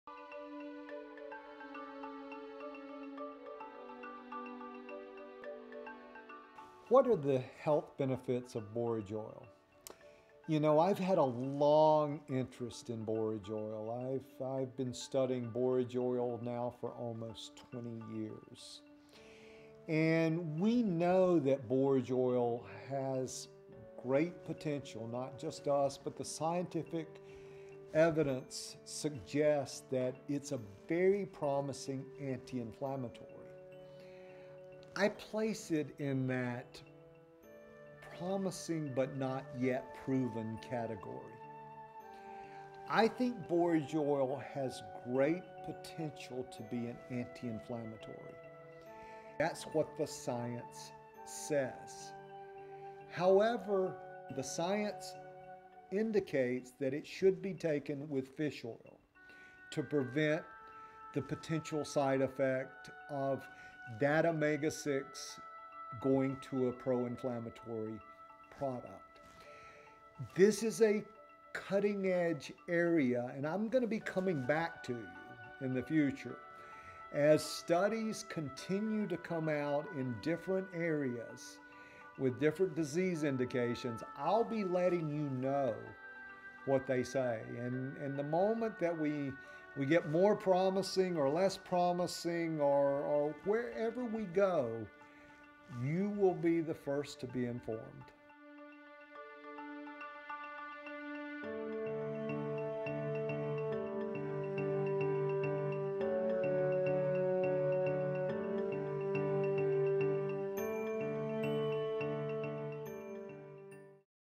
What Is Borage Oil? | Dr. Ski Chilton
Dr. Ski Chilton discusses the science of borage oil, its health benefits, and its potential as an anti-inflammatory.